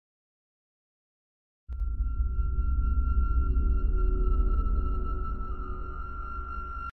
giraffe carrot knife peoples
Try not to laugh with this meme here. Mysterious meme in this video.
Animal meme with giraffe peeking through the door.